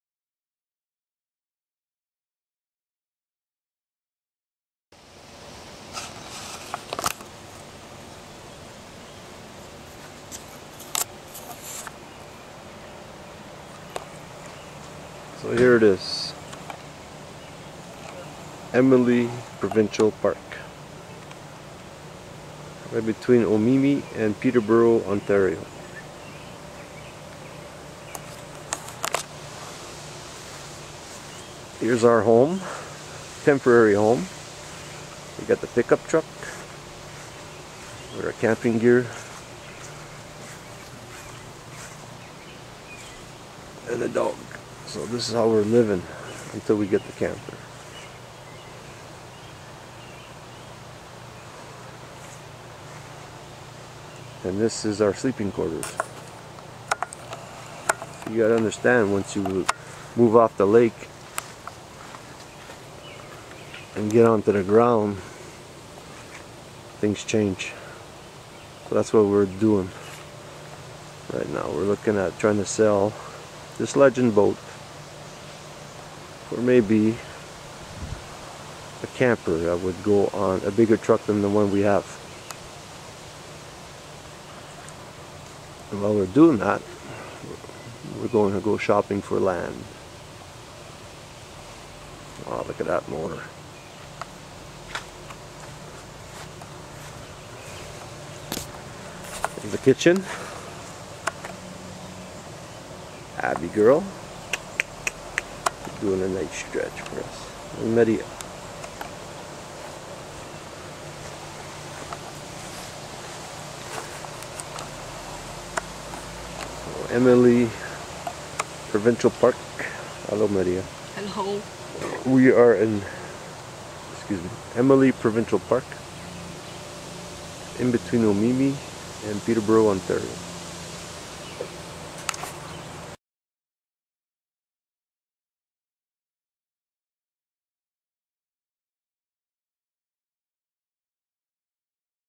Boat Campers
An 18 foot Legend Xcalibur aluminum boat was used as a sleeping tent while camping in the Kawartha Lakes area in Ontario Canada.  It was very comfortable but a challenge to climb into without a step latter.

The Kawartha Lakes area of Ontario is littered with lakes and waterways to launch a watercraft for sporting.  Finding a campground is no challenge either, since there are host of parks in this area.